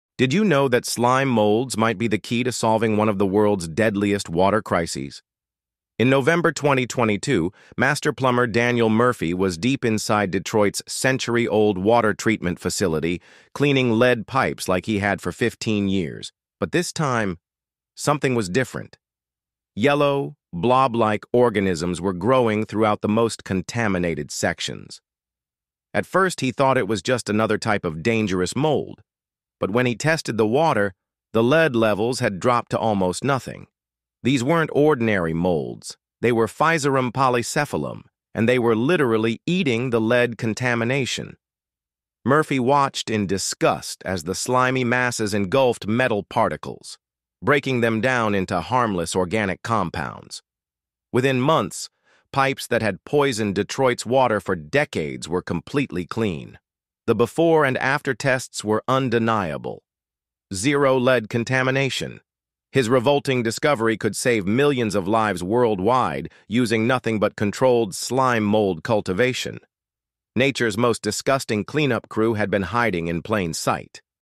Plumber Discovers Slime Molds That EAT Lead From Water Pipes
Slime molds cleaning lead pipes better than chemicals 🤯